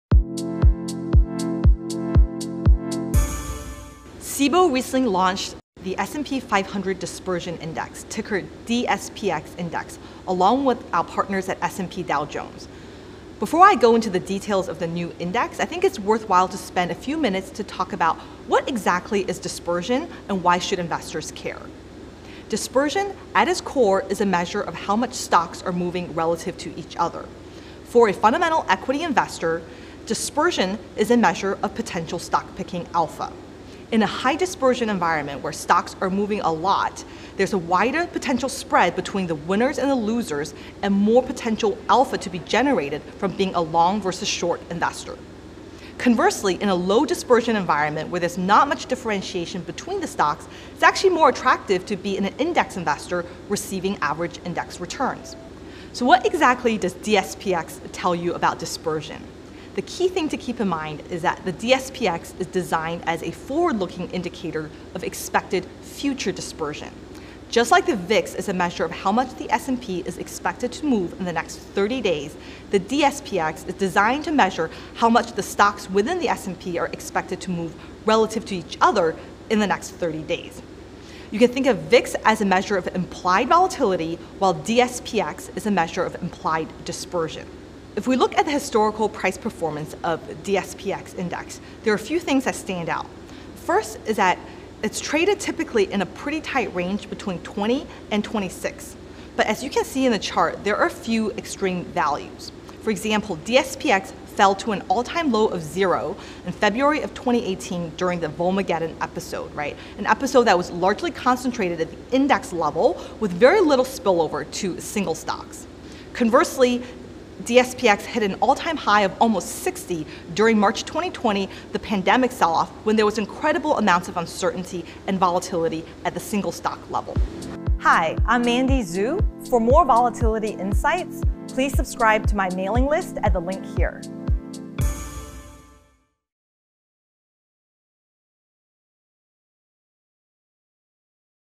Volatility Insights with Mandy Xu - DSPX Dispersion Index
What do market participants need-to-know about the Cboe S&P 500 Dispersion Index (DSPX)? In this Volatility Insights clip, Mandy Xu breaks down distinctions between high and low dispersion environments, the VIX and DSPX Indices and more.